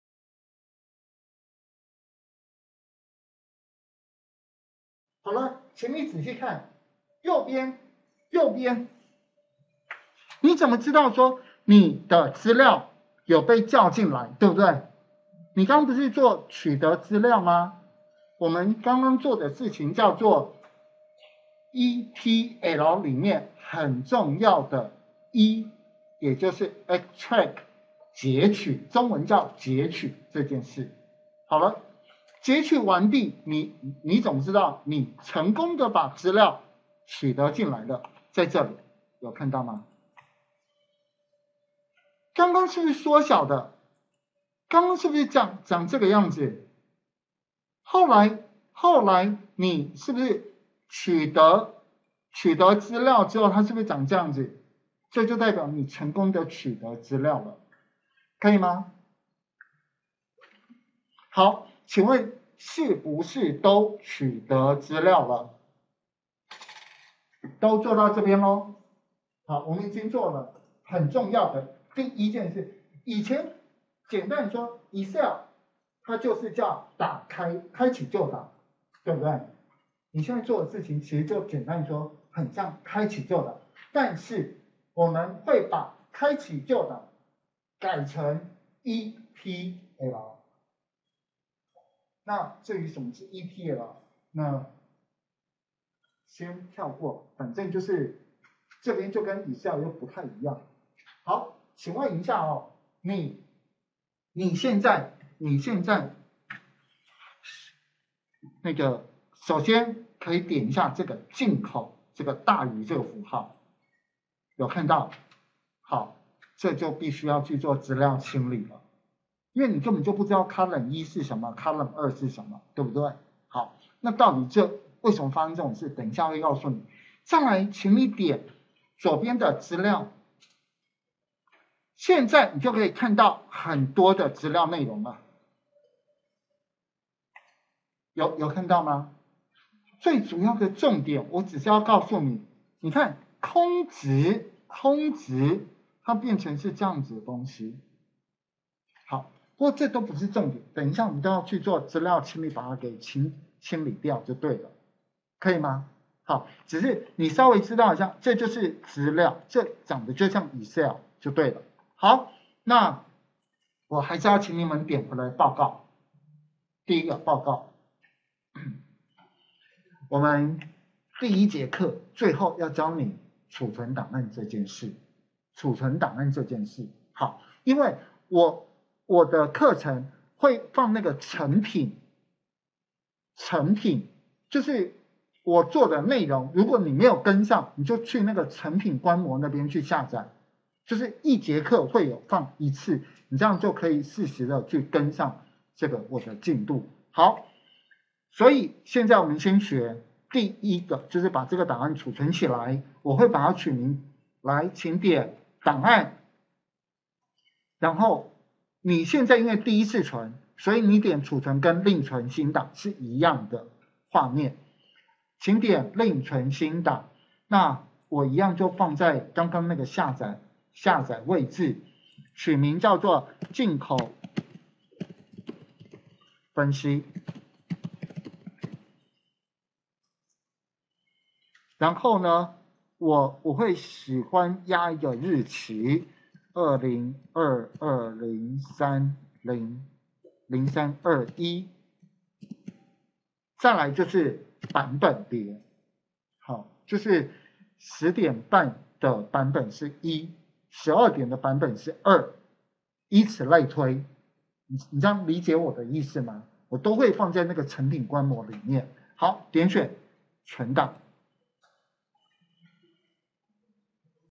11 檢視載入資料並儲存-資料視覺化數據分析工具應用
載入後，檢視資料欄位均為Column1、column2. 先確認載入的資料無誤後，存檔。